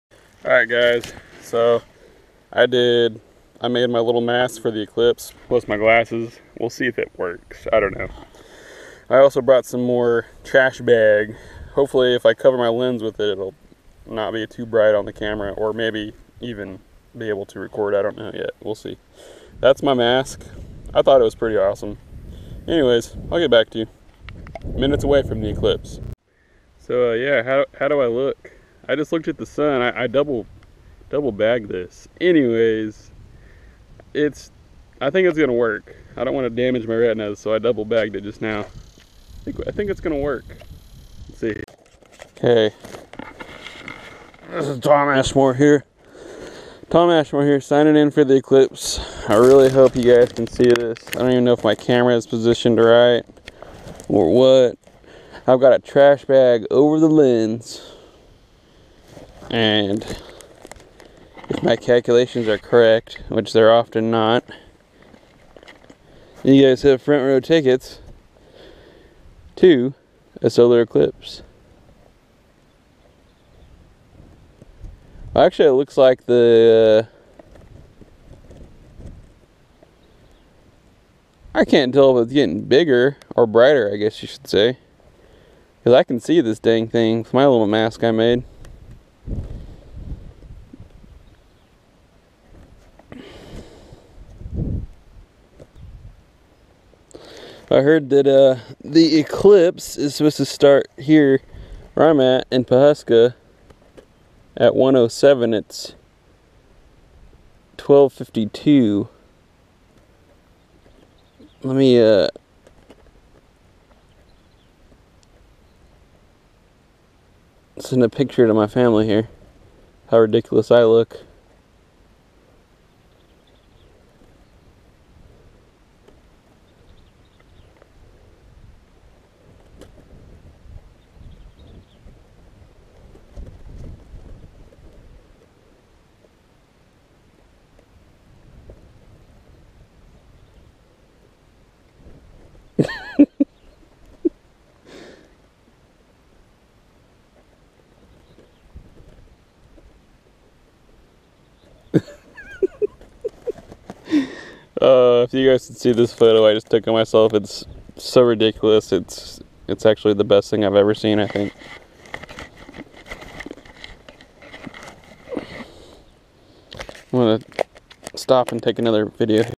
Solar Event feat. ECLIPSE MAN
This was fun and DEFINITELY goofy... Didn't get the event quite as well as I had hoped but oh well...catch it again in 2024 maybe...